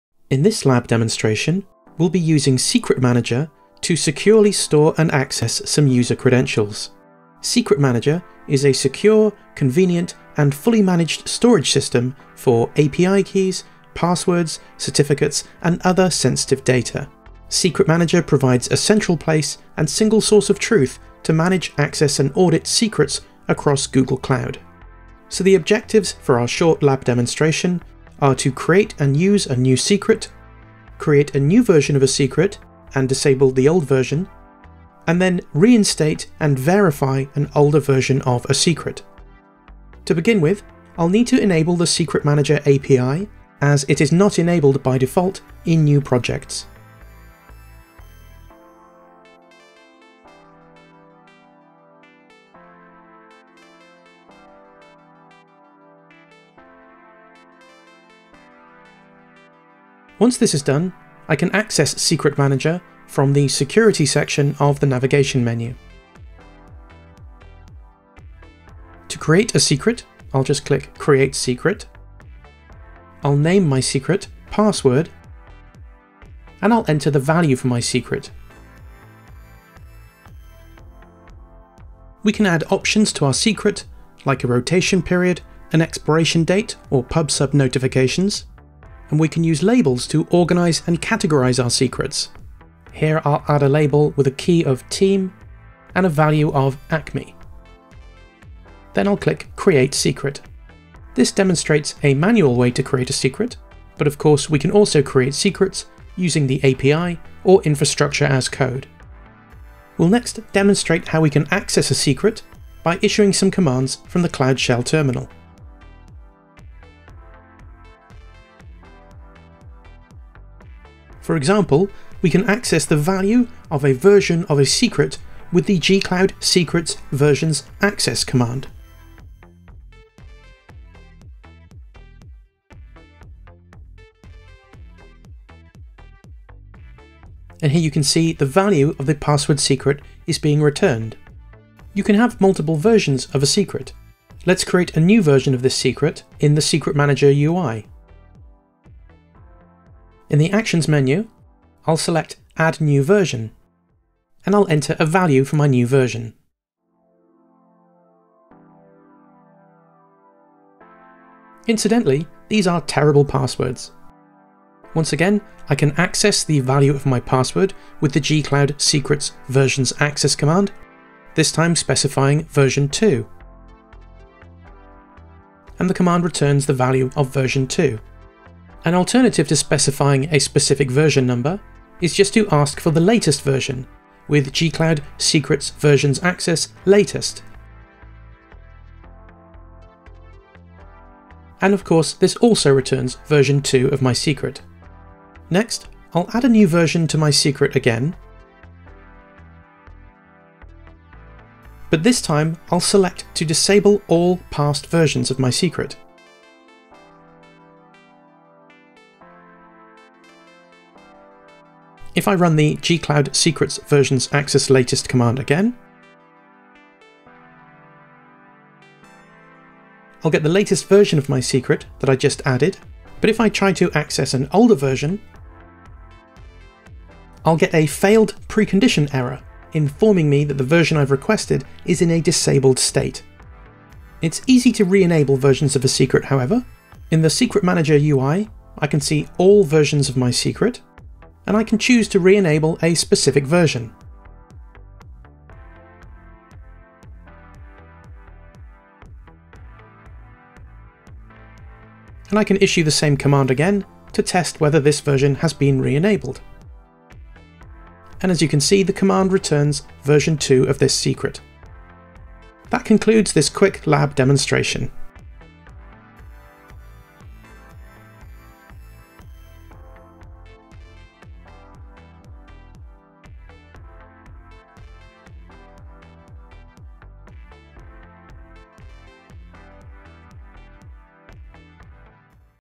Google Cloud Secret Manager quick introduction
In this quick lab demo, we're going to experiment with Secret Manager, Google Cloud's fully managed service for storing API keys, passwords, certificates, and other sensitive data.

In this video, we will create a secret and see how we can access it using command line tools, as well as manage different versions of secrets.